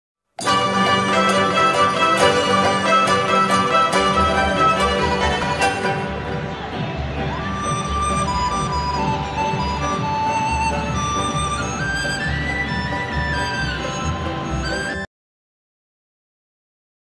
jade art
linyi pullman hotel showing.
Excellent grinding technology.